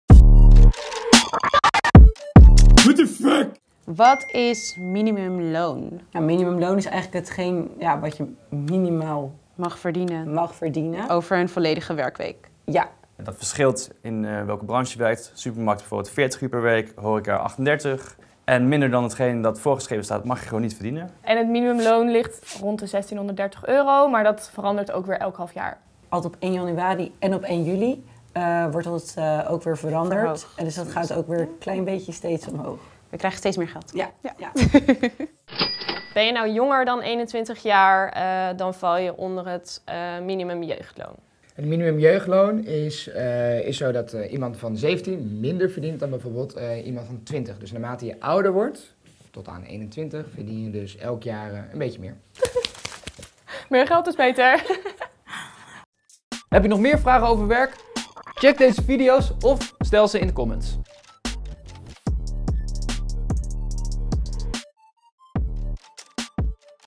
Wat is minimumloon? - What the FAQ?! [#1]
In deze video:
0:00 Wat is minimumloon?
0:20 Hoe hoog is het minimumloon?
0:35 Wat is het minimumjeugdloon?

Minimumloon: dat is hetzelfde voor iedereen en verandert elk halfjaar. Wat het minimumloon is, zie je in deze video. Hier leggen de recruiters van uitzendbureau YoungCapital binnen één minuut uit welk bedrag voor jou geldt.

In de YouTube-serie What the FAQ geeft YoungCapital antwoord op al jouw werkvragen. Deze keer nemen we het minimumloon onder de loep. Dit is een vast bedrag waar iedereen in Nederland minimaal recht op heeft als hij of zij aan het werk gaat. Hoe hoog dit bedrag is, hangt onder andere af van je leeftijd en hoeveel uur je werkt per week.

Wat nog meer invloed heeft op jouw minimumloon? De sector waarin je werkt en eventuele cao-afspraken. Zo moet je 40 uur in een supermarkt werken om hetzelfde minimumloon te krijgen als iemand die 38 uur in de horeca werkt. Belangrijk dus om bij jouw werkgever te checken welk minimumloon voor jouw sector geldt.

Ook goed om te weten: vaak wordt het minimumloon aangegeven in bruto-bedragen. Houd er dus rekening mee dat er nog belasting afgaat, en dat het minimumloon dat je overgemaakt krijgt lager is dan staat aangegeven. Kun je wel een kleine opfrisbeurt gebruiken over hoe het ook al weer zit met bruto- en nettoloon? Check dan deze video waarin onze recruiters het allemaal op een rij zetten.